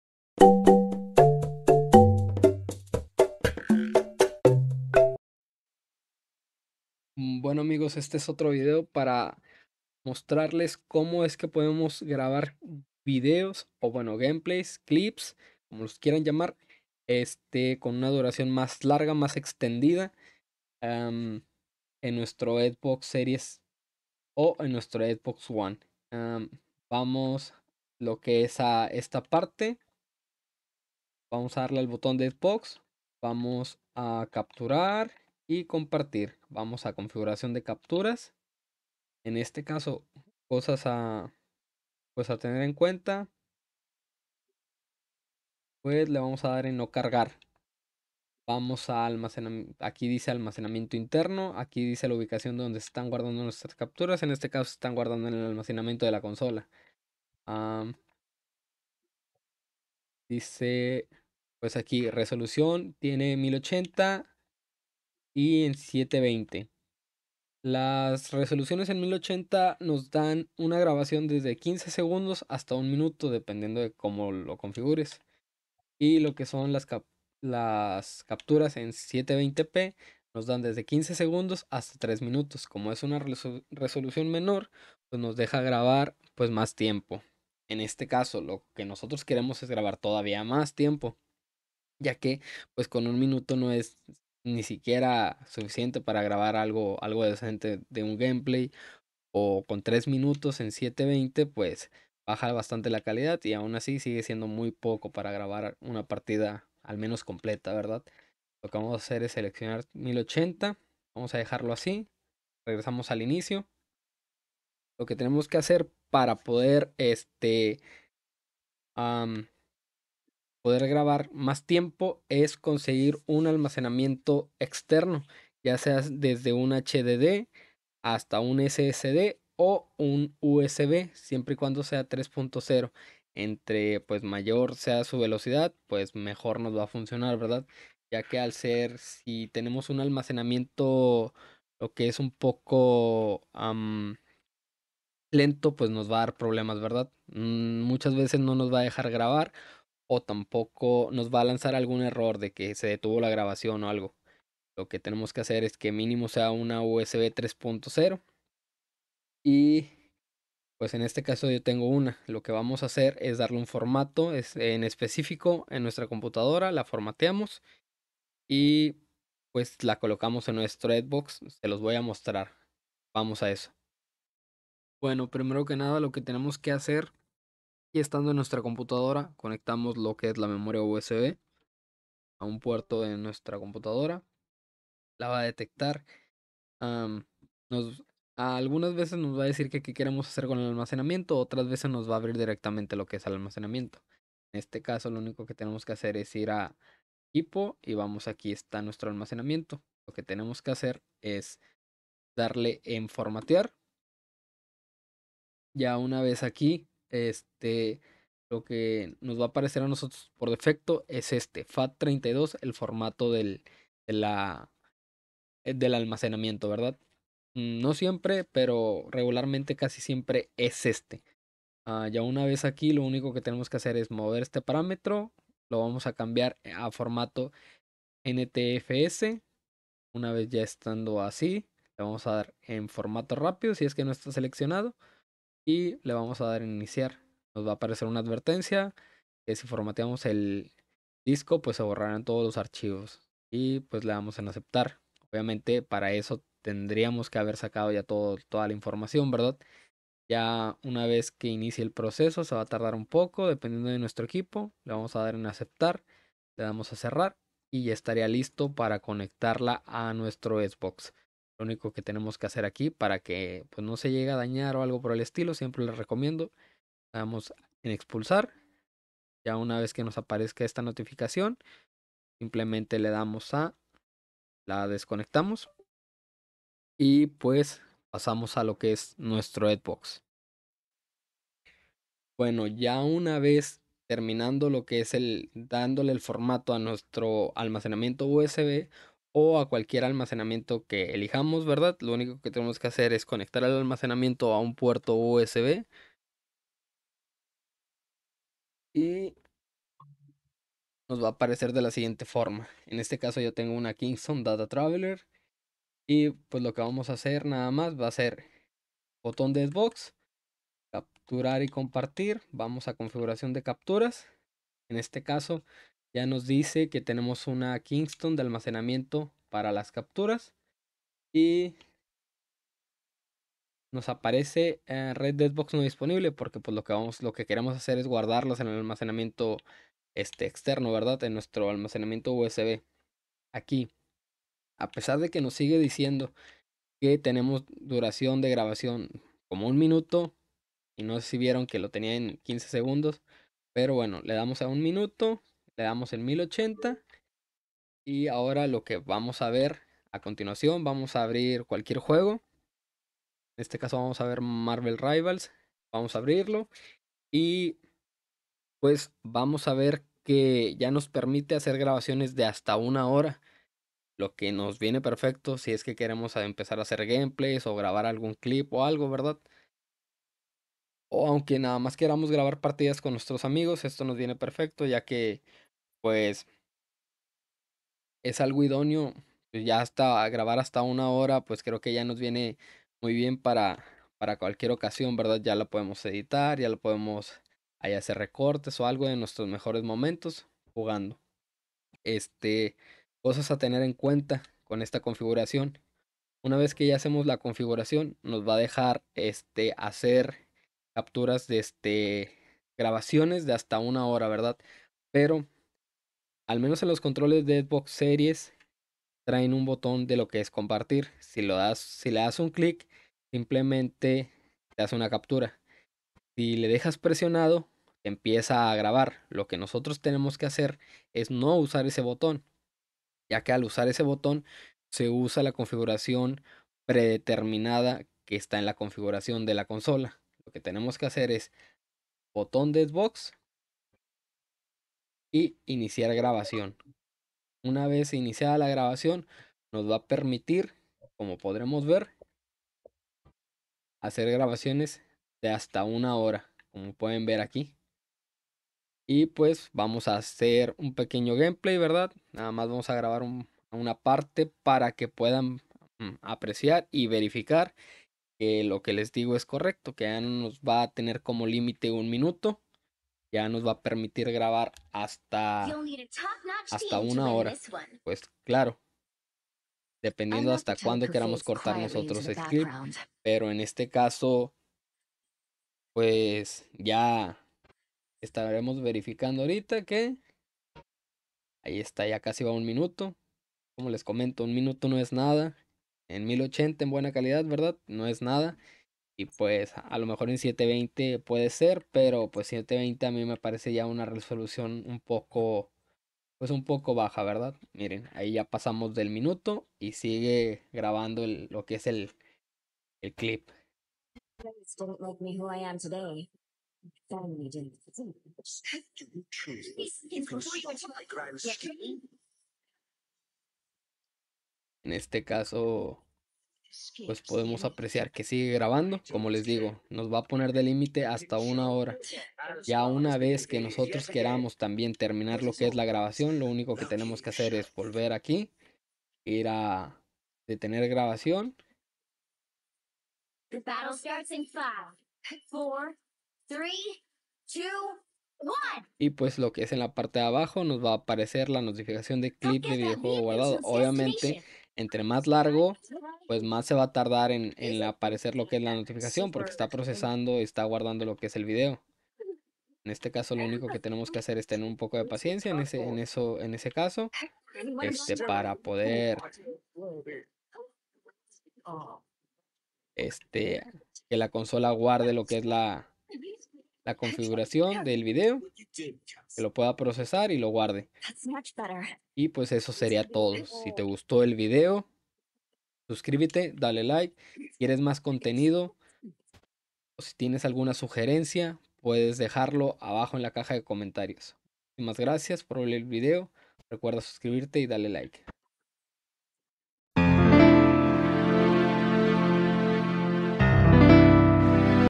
Grabar Clips Con Mas Duración En Xbox Series S/X (Super Facil)
en este video les muestro como poder configurar nuestra xbox para poder grabar videos con mayor duración y así puedan obtener sus mejores momentos para lo que los necesiten

MARCAS DE TIEMPO:
0:00 Grabar Clips Con Mas Duración - Intro
0:11 Configuraciones preestablecidas y requisitos
2:53 Formato correcto de almacenamiento
4:42 Configuración correcta y recomendaciones